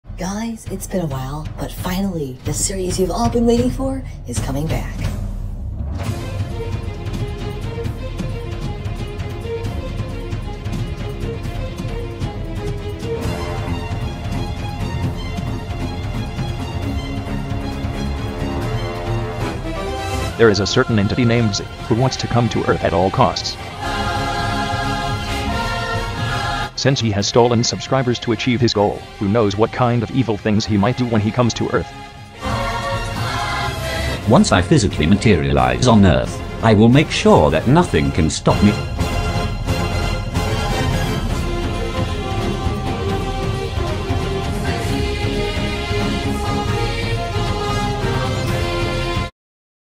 The Legend of Z Continuance
Trailer for the Continuance of the Legend of Z Series animation, featuring the animated short film with titular character such as the energy being named Z and the famous FBI agent named Randolph Foster and Mister Best
It's been about two years since I started Z (two years ago July and now it is July again)